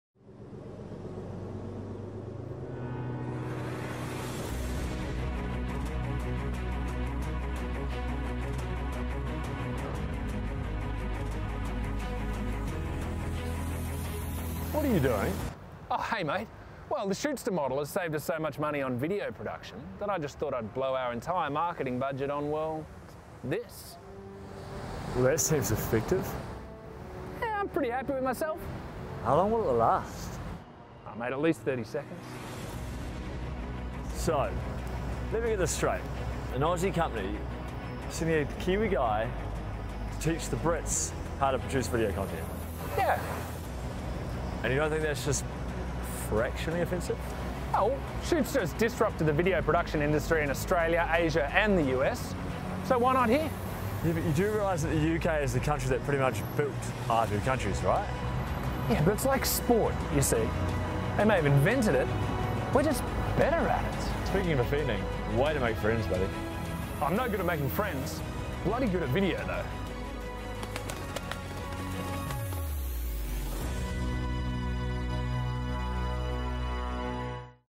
Shootsta Launches in the UK
Shootsta has finally made it to the UK! Our innovative business model has continued to change the video production landscape from Australia, Asia, USA – and now we're about to change it in the UK and Europe!